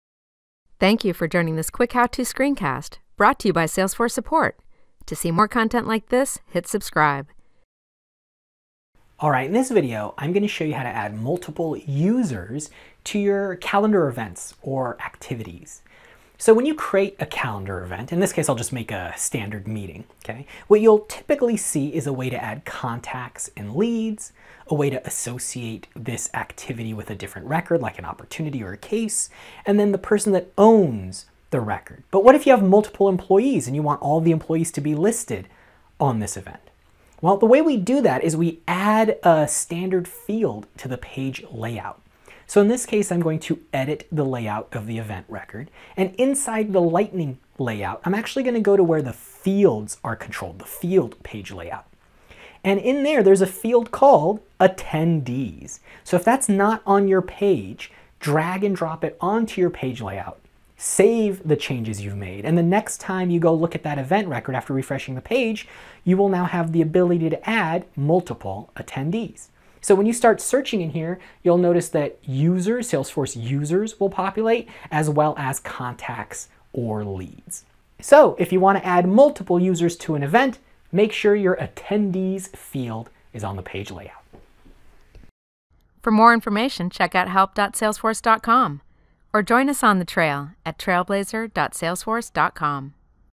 How to Add Multiple Users to an Event | Salesforce Platform
After watching this screencast the user will be able to learn how to add multiple users to an event.